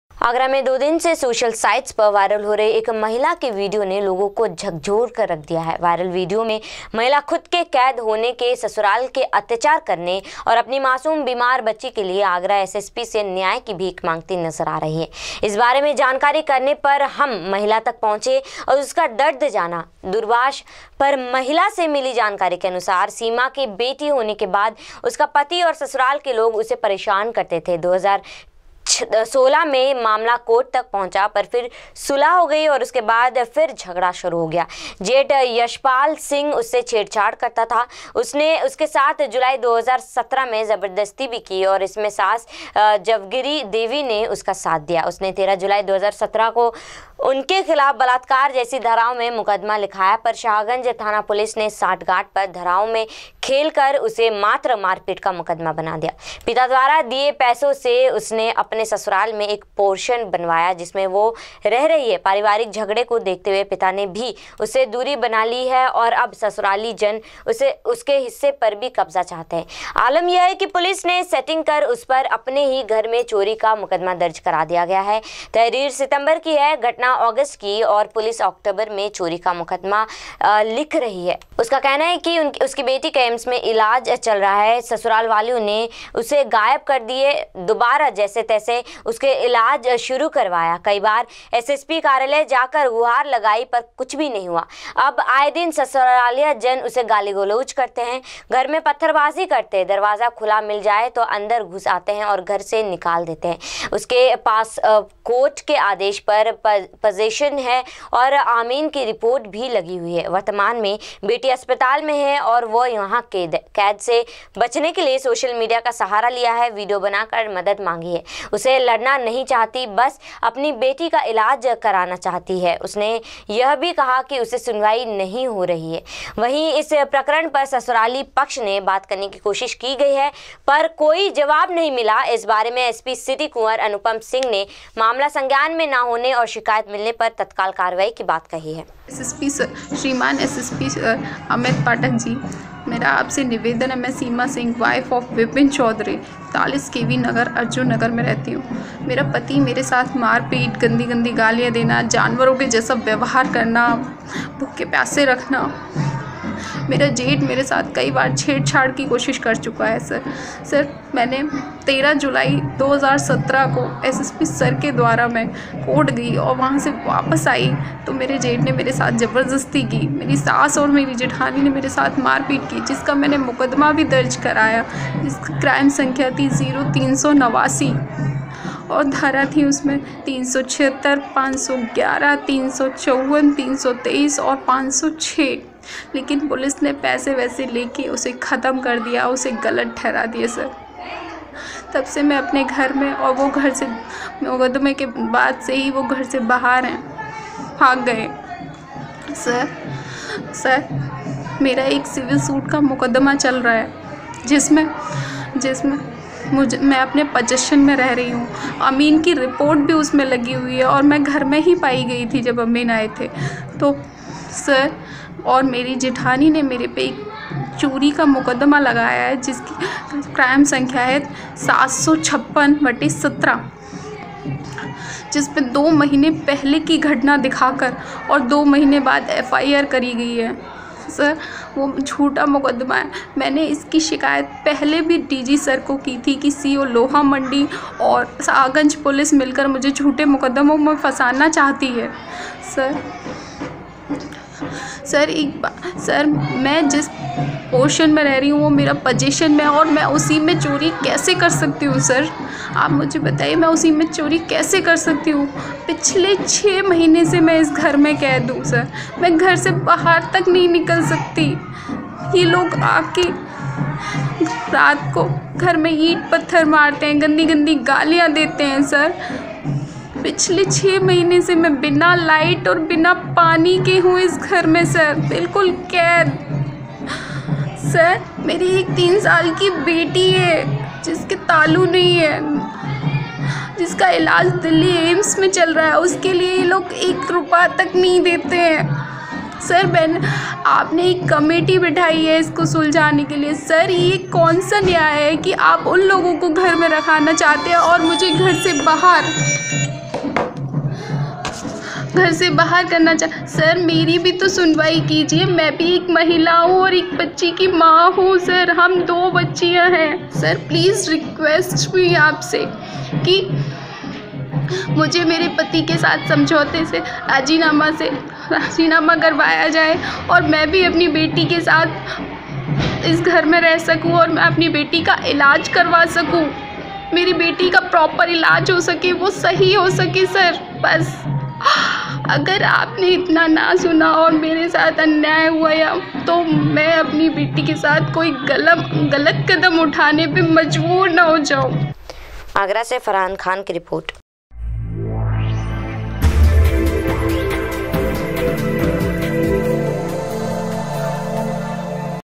[Agra News] वायरल हो रहे एक महिला के विडियो ने लोगो को झकझोर रखा / THE NEWS INDIA
आगरा में दो दिन से सोशल साइट्स पर वायरल हो रहे एक महिला के विडियो ने लोगो को झकझोर रखा था|वायरल विडियो में महिला खुद के कैद होने ससुरालियो के अत्याचार करने और अपनी मासूम बीमार बच्ची के लिए आगरा एसएसपी से न्याय की भीख मांगती नजर आ रही थी|इस बारे में जानकारी करने पर हम महिला तक पहुंचे और उसका दर्द जाना